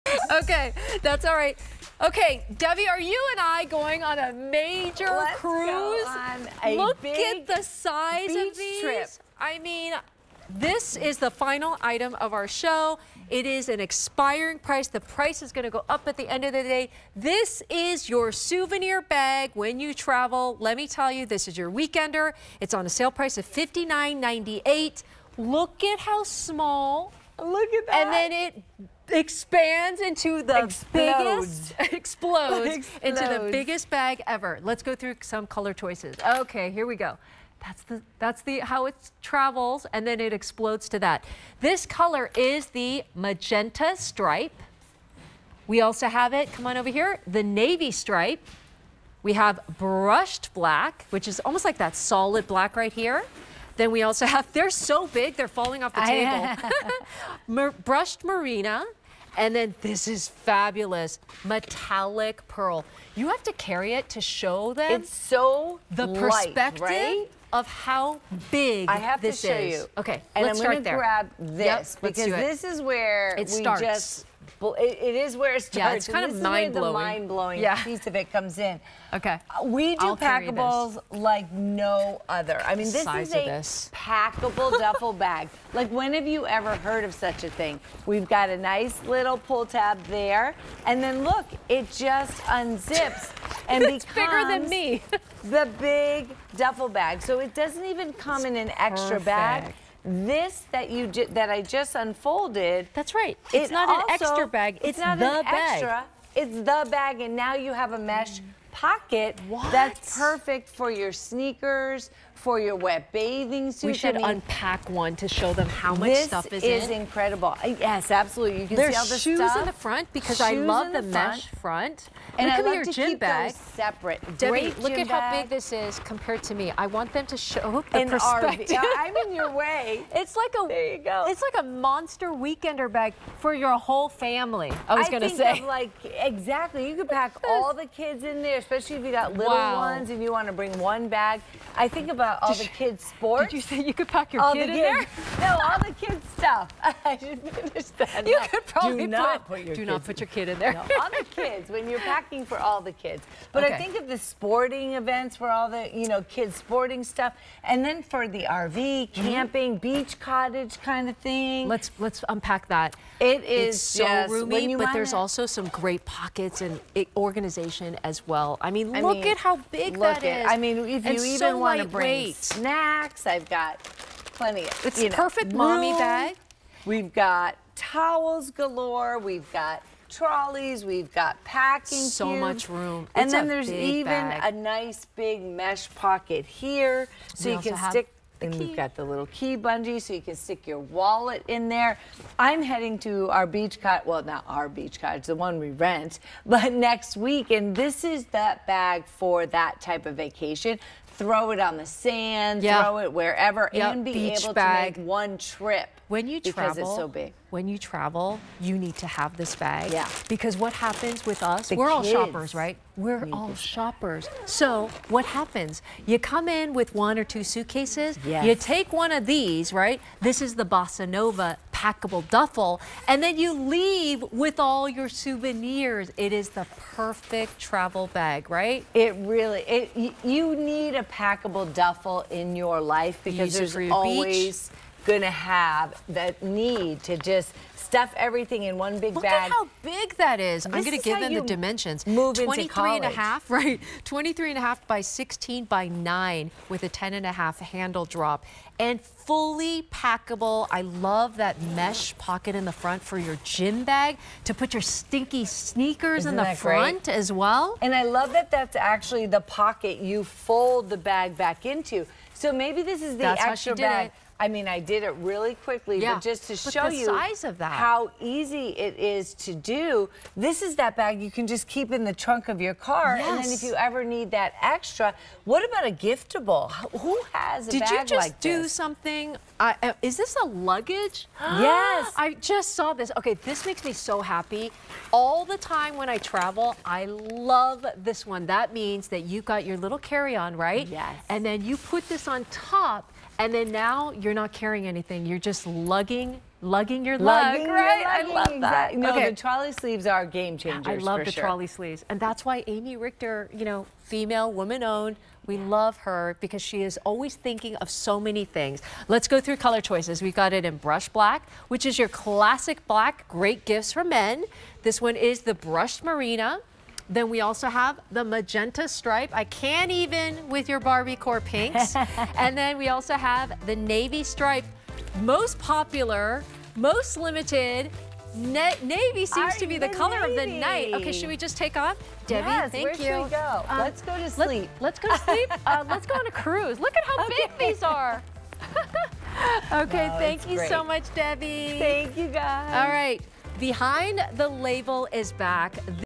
Lug Packable Duffel - Bossa Nova on QVC
Lug Packable Duffel - Bossa Nova

A travel must-have, this large duffel bag is great for overnight trips, weekend getaways, or longer excursions. The mesh pocket allows for easy access to essentials (and it's a great place to store wet items). Fold it into a pouch when not in use. From Lug.